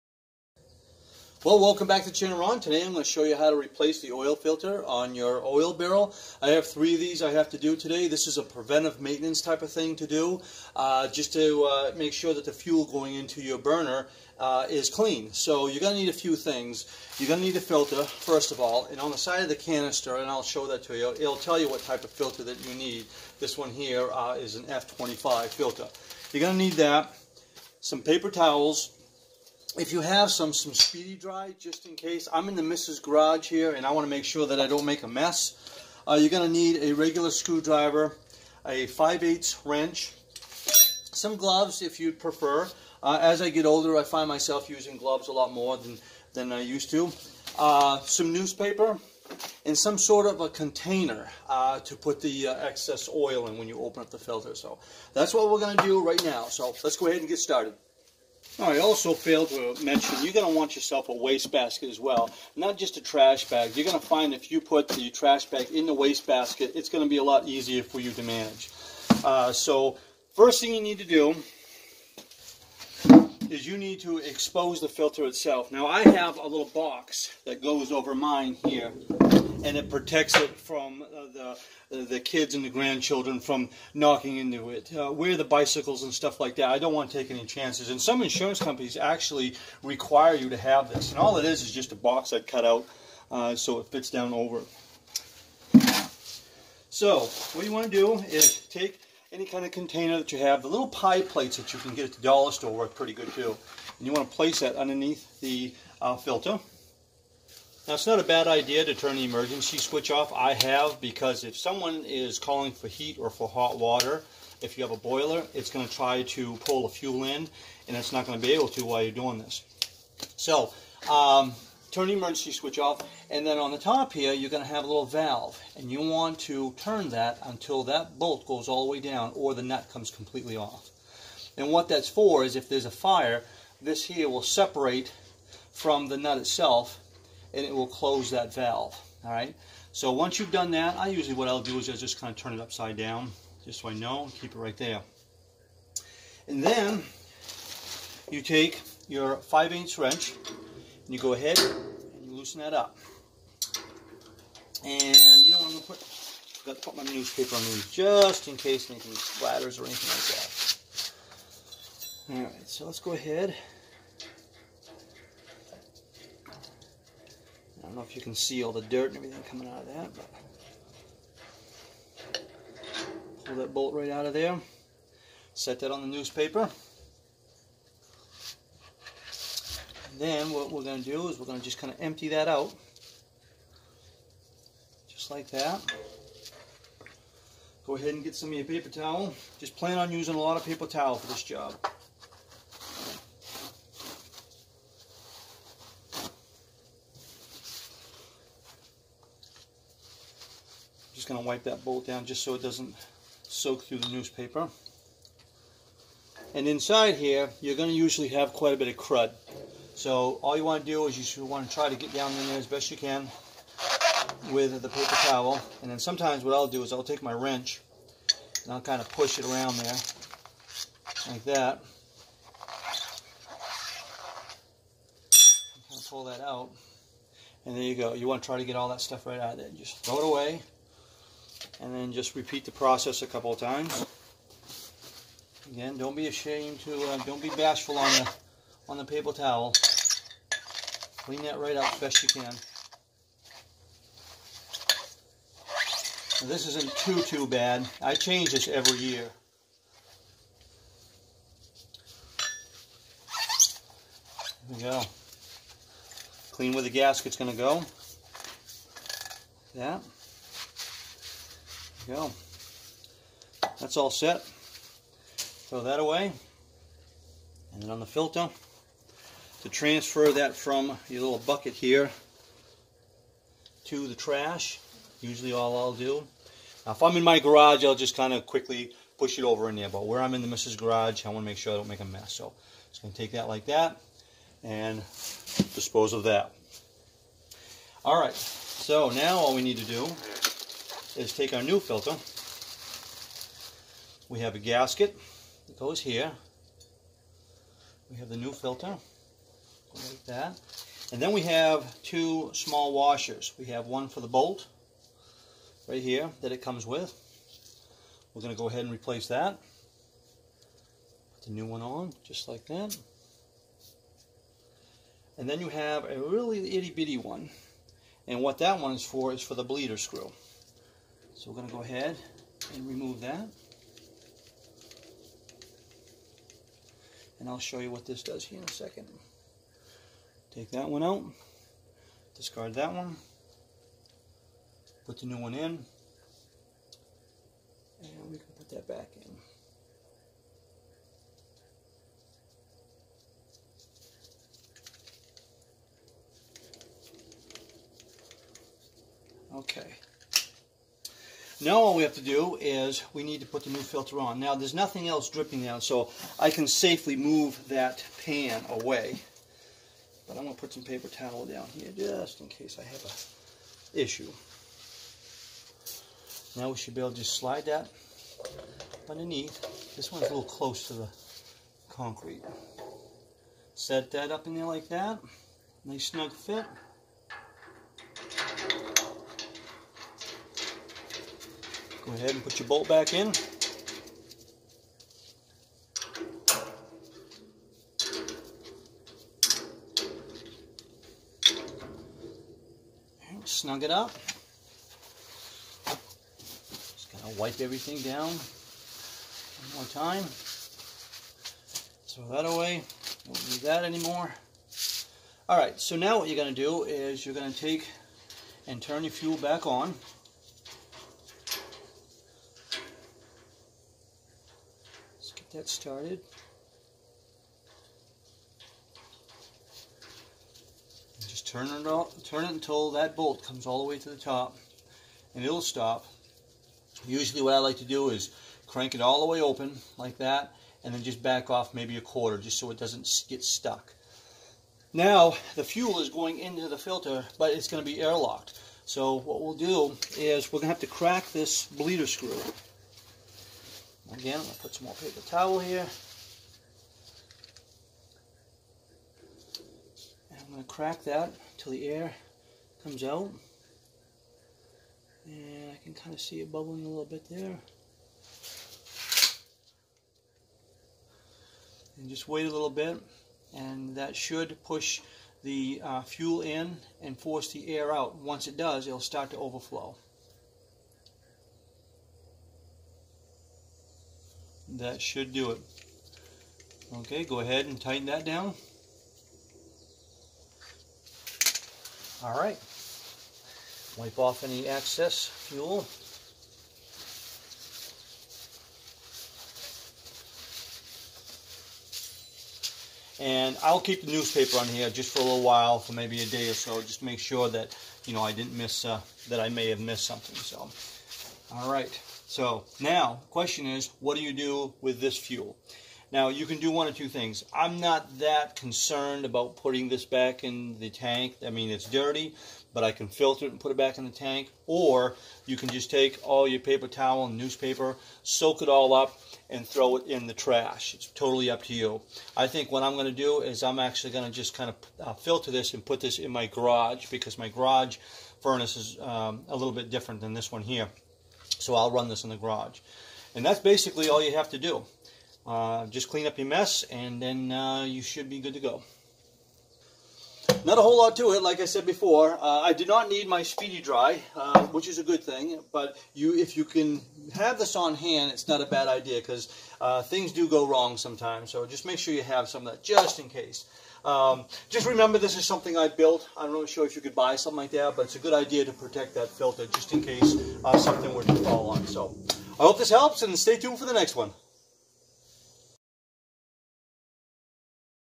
How To Change Filter On Your Oil Tank
How To Change  Filter On  Your Oil Tank: Colder weather is getting closer and I like to change them myself, very easy to do, cost of filter $5 to 10 depending where you go.

Parts:
Oil filter

 Tools Used:
Kobalt Tool Set

Film Gear Used:
IPad Air
Soft-box Lights

Editing Equipment:
Dell Latitude E6420 Laptop
           powerdirector app